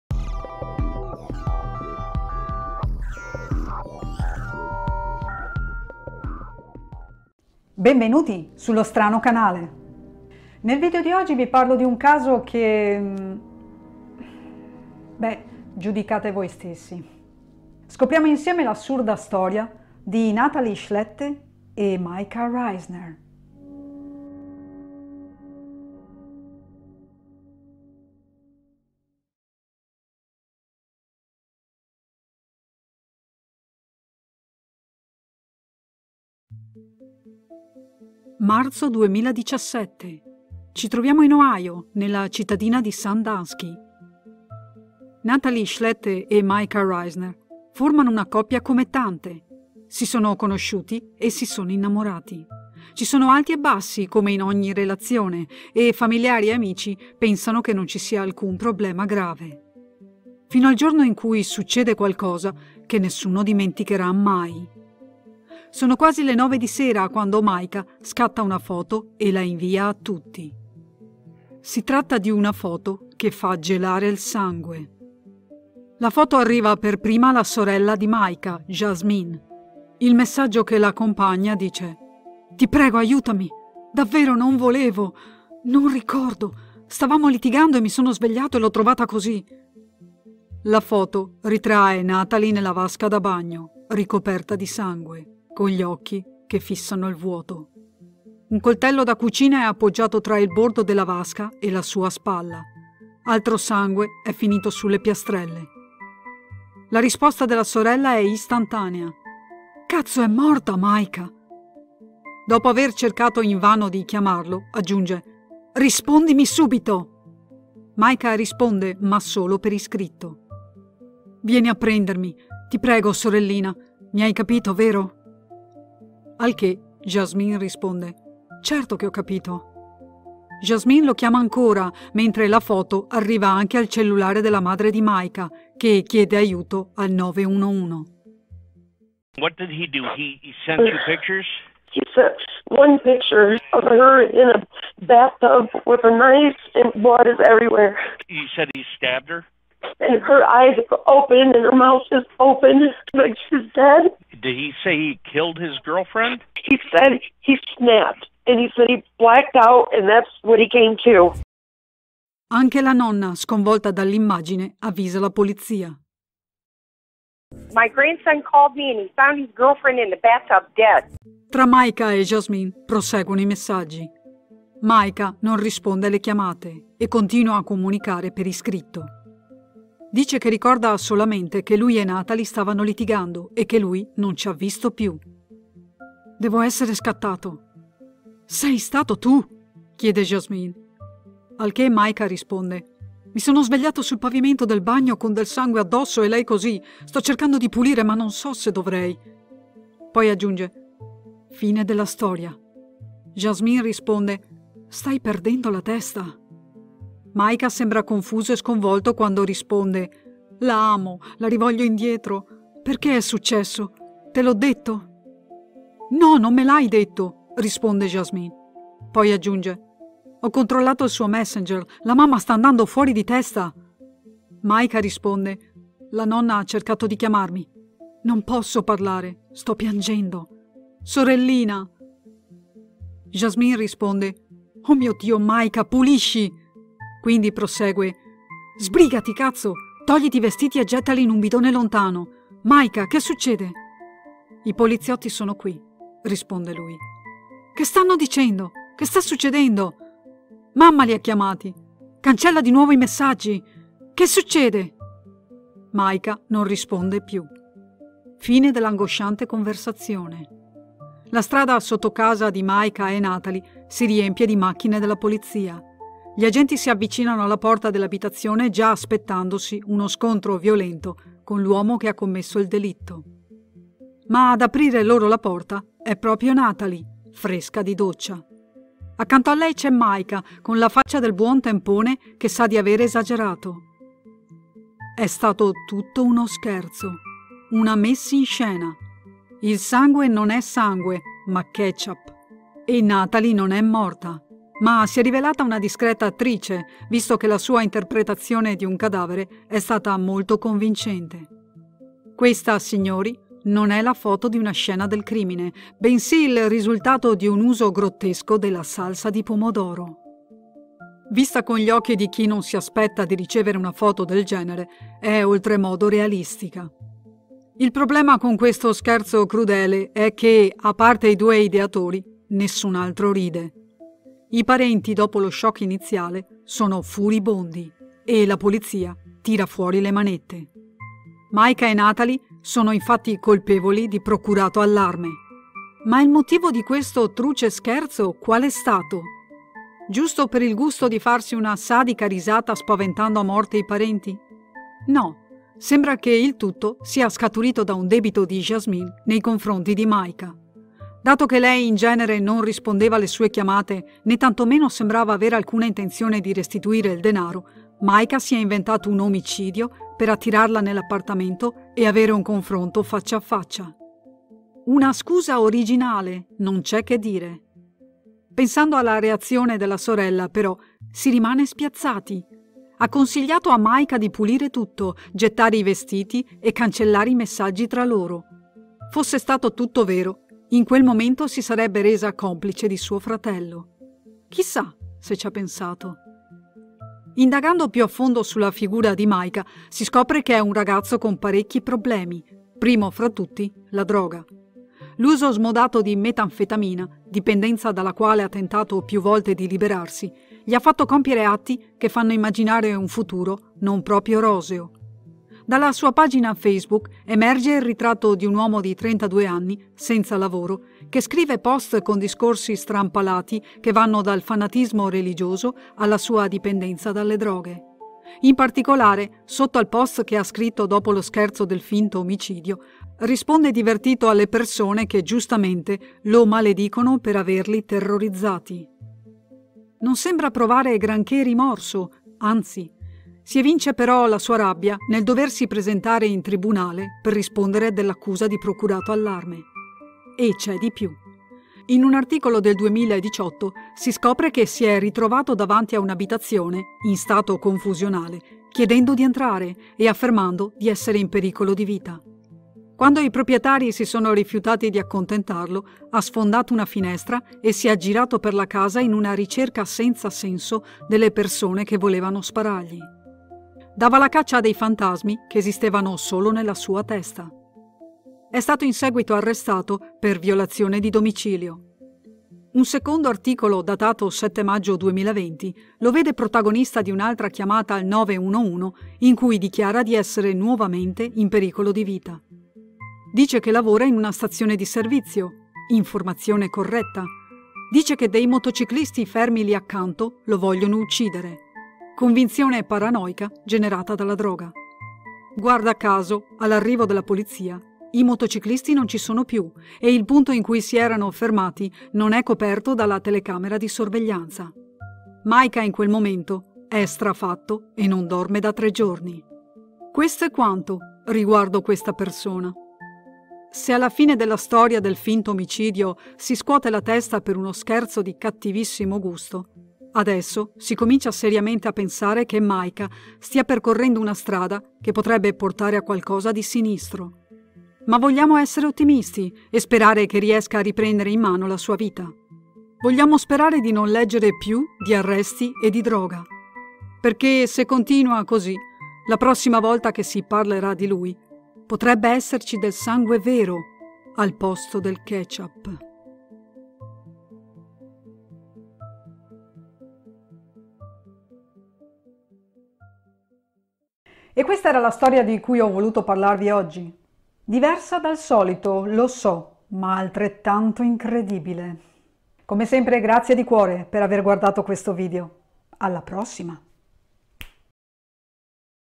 L'ASSURDO CASO DI NATALEIGH SCHLETTE E MICAH RISNER
Ciao! Nel video di oggi vi voglio parlare di un caso diverso dal solito, ma capirete cosa intendo solo dopo averlo visto.
Grazie di cuore per aver guardato questo video!

📷 Immagini: tutte le immagini appartengono ai legittimi proprietari.
🎥 Eventuali brevi spezzoni di telegiornali/interviste/interrogatori sono utilizzati a puro scopo informativo e d'intrattenimento.